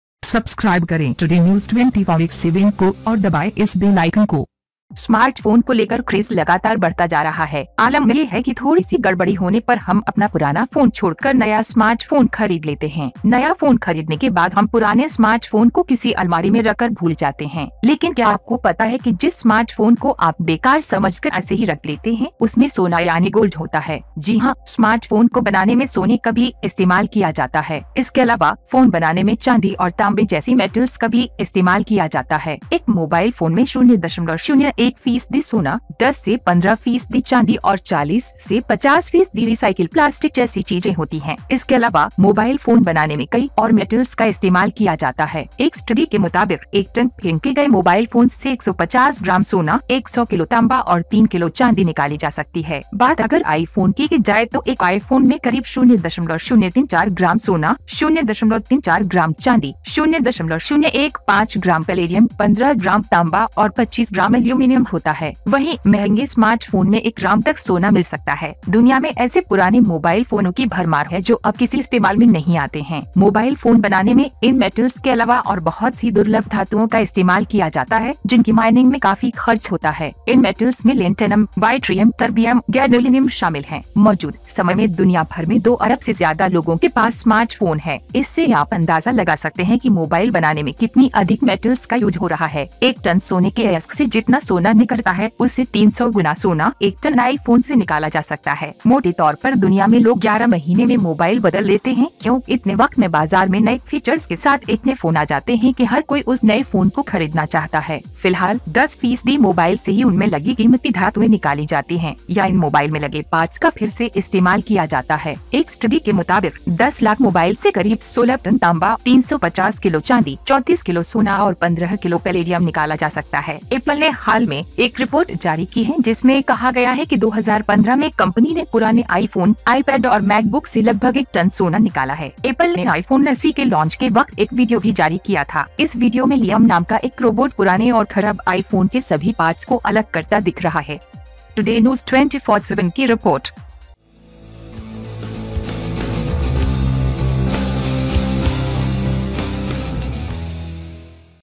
फेंके नहीं अपने पुराने mobile phones और computers इनमें छिपा होता है gold
Craz is constantly growing on the smartphone. Alam is that we have to leave our old phone and buy a new smartphone to get a little disturbance. After buying a new phone, we forget the old smartphone in a closet. But do you know that the smartphones that you consider to be worthless, keep gold in it? Yes, gold is also used in making smartphones. In addition, silver and copper-like metals are also used in making phones.

A mobile phone has things like 0.01 percent gold, 10-15 percent silver and 40-50 percent of recycled plastic. In addition, many more metals are used to make mobile phones. According to a study, 150 grams of gold, 100 kg of copper and 3 kg of silver can be extracted from a tonne of mobile phones.

If the iPhone goes, then an iPhone contains 0.034 grams of gold, 0.34 grams of silver, 0.015 grams of palladium, 15 g copper, and 25 grams of aluminum.

At the same time, expensive smartphones can get gold up to one gram. There is a lot of old mobile phones in the world which are no longer used.

In addition to these metals, many rare metals are used in making mobile phones, which have huge expenditure in mining. These metals include Testamentum, cerium, terbium, and gadolinium.

At present, more than two billion people have smartphones around the world. This can help you to figure out how many more metals are being used to create mobile. Gold as much as one ton of gold ore leaves, three hundred times gold, one ton can be removed from the iPhone.

Broadly, people change the world in 11 months, because in such a long time there are so many phone calls with new features in the market that everybody wants to buy that new phone.

Right now, the precious metals used in them from 10 percent of the mobile are removed or the parts used in these mobile phones are used again. According to a study, about 16 tonnes of copper, 350 kg silver, 34 kg of gold and 15 kg palladium can be extracted from one million mobile phones.

Apple has recently released a report stating that in 2015, the company has brought about 1 ton of gold from the old iPhone, iPad, and MacBook. Apple also released a video during the launch of the iPhone SE. In this video, a robot named Liam appears to split all the parts of the old and bad iPhone.